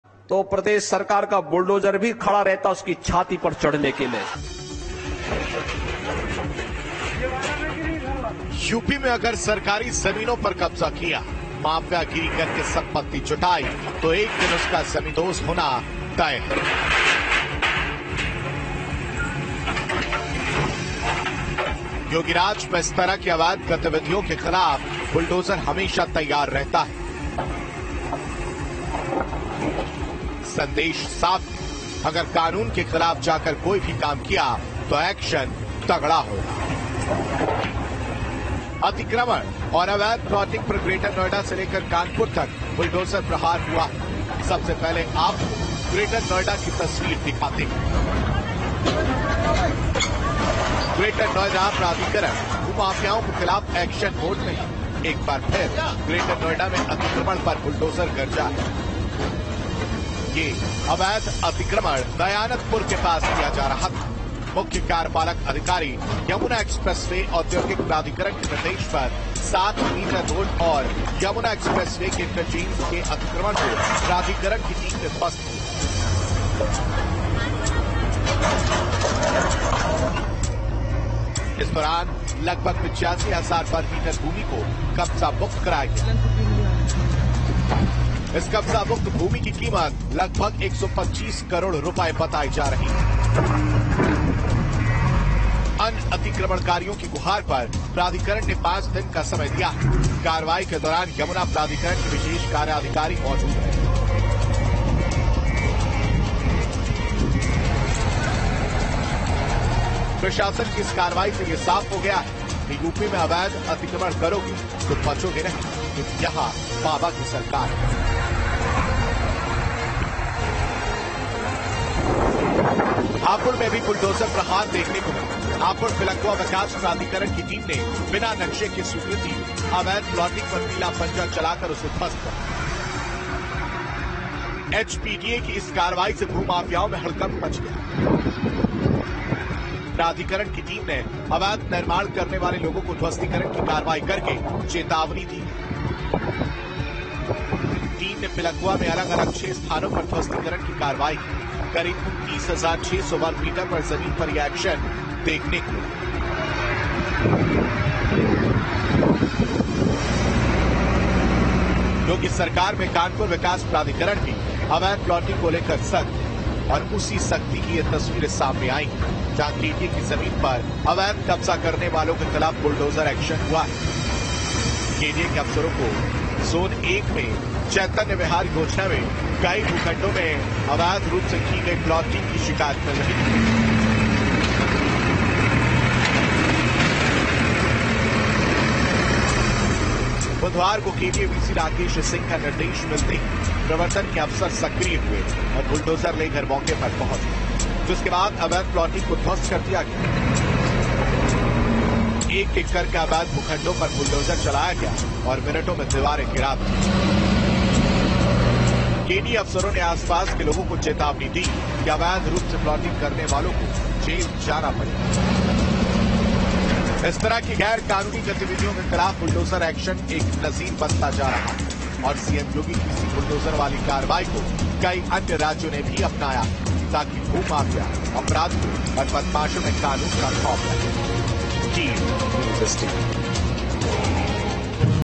Bulldozer Action: Kanpur में अवैध कब्जे पर चला KDA का बुलडोजर | Breaking News | Hindi News
News State Apps डाउनलोड करें:
Download Android and iOS Apps for more updates: